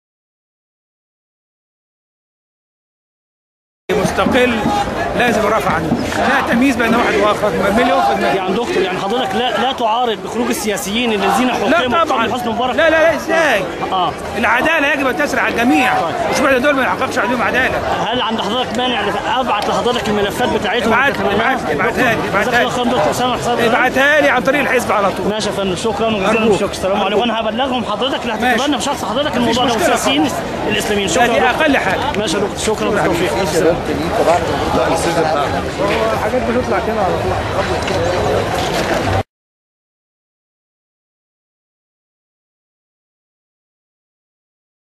أسامة الغزالى حرب يتبنى قضية المعتقلين السياسيين
من وسط ميدان التحريرفى يوم الثلاثاء سجلت شبكة يقين الإخبارية حوار مع أسامة الغزالى حرب المرشح لرئاسة الجمهورية تبنى فيه قضية السياسيين الذين حُكموا فى ظل النظام السابق وما زالوا فى السجون حتى الأن